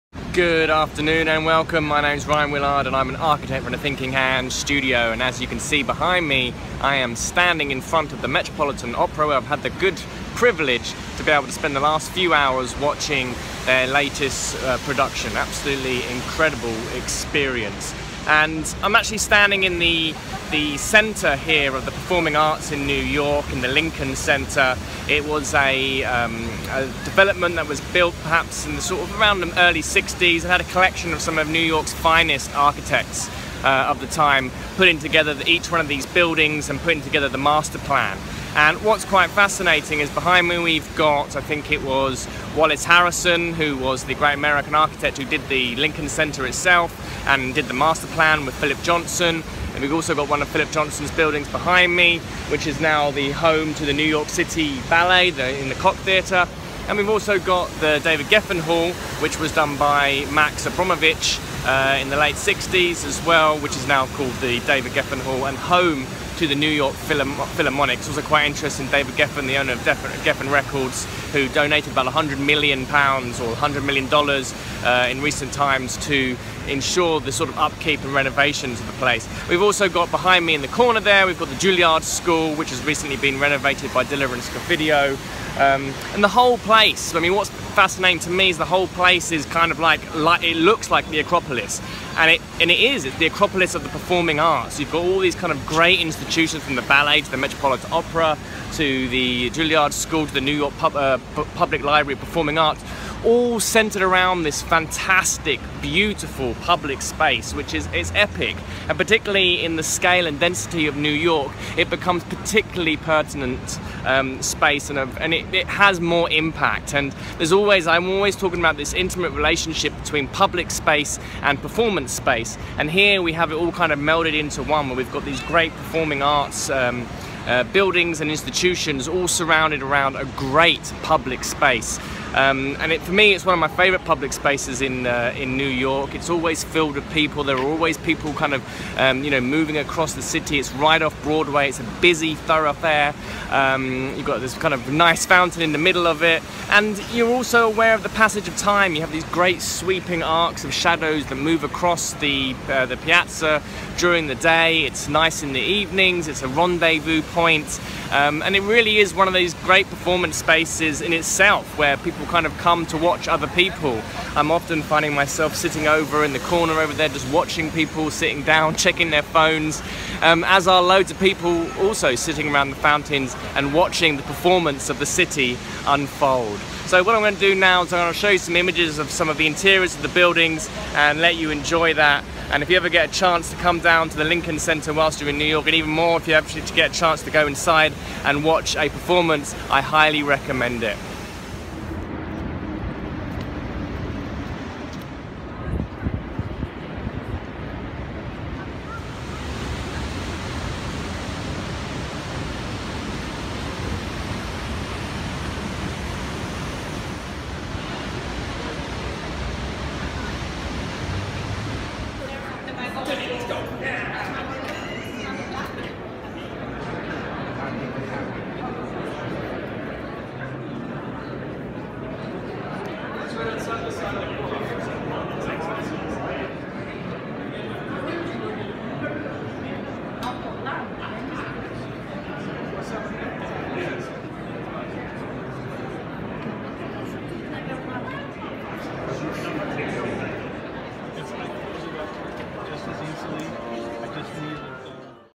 The Lincoln Centre, Broadway, New York City
The Acropolis of the performing arts in New York City is the Lincoln Centre now home to some prestigious institutions such as the New York Philaharmonic, the Metropolitan Opera and the New York Ballet. A brave and dramatic architectural complex featuring works from some of America's great architects of the time such as Philip Johnson, Wallace Harrisom and Max Abramovic. Centre around an epic piazza the Lincoln Centre represented a move towards prosperity and growth and marked a moment in time after the Second World War where such projects had not been undertaken. Here Rion takes a quick visit to the complex after a performance at the Metropolitan Opera.